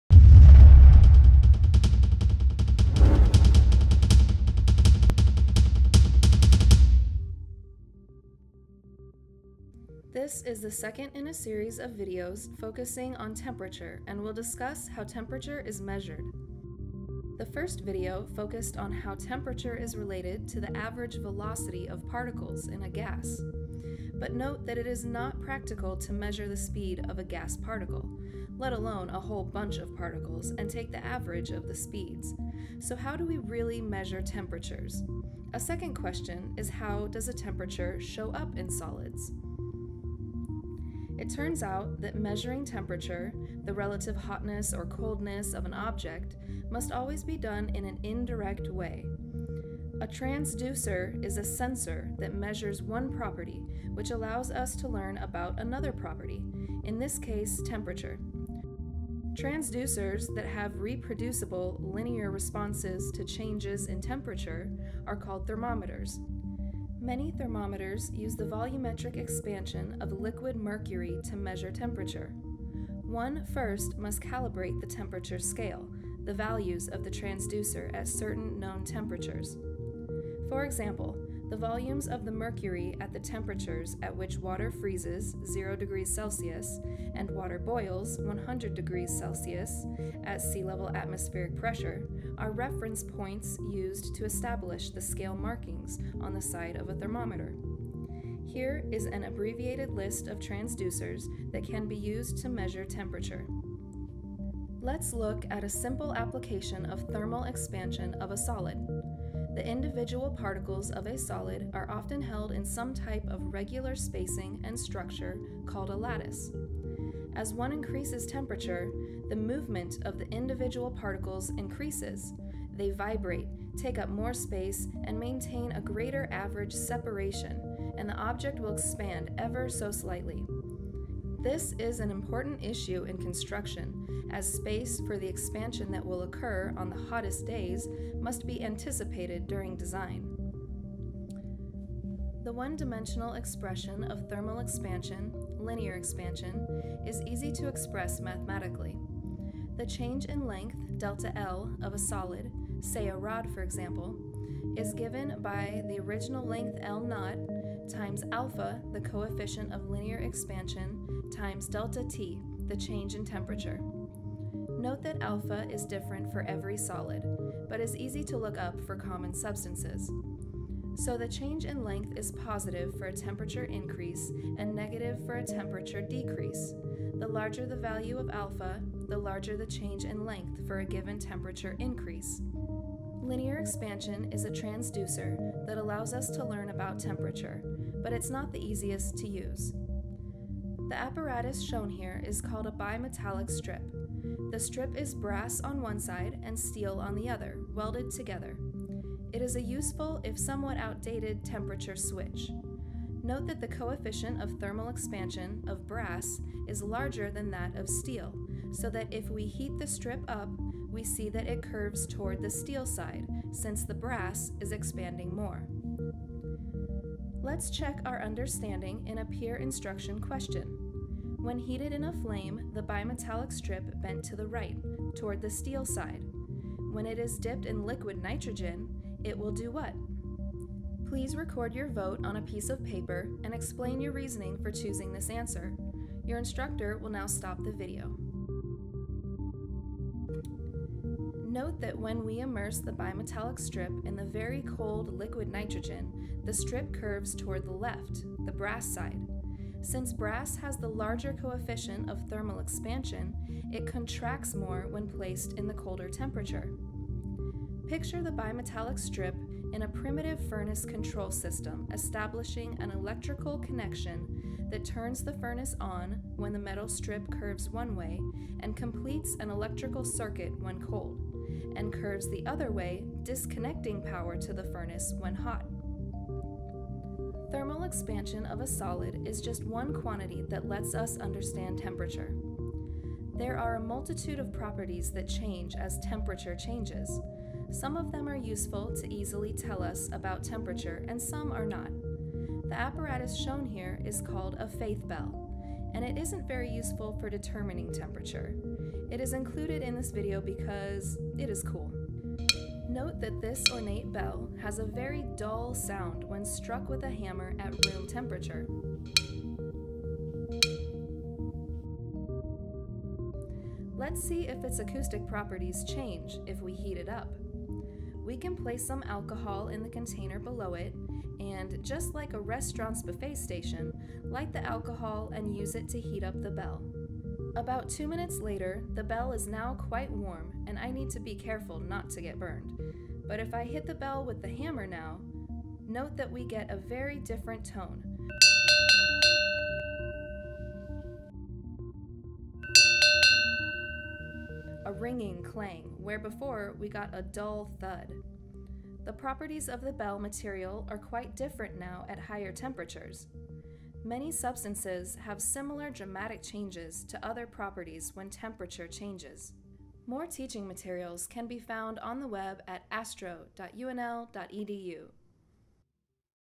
Temperature 2
This video continues the discussion of how we measure temperature and will look generally at the effects that temperature changes have on solids and specifically at thermal expansion.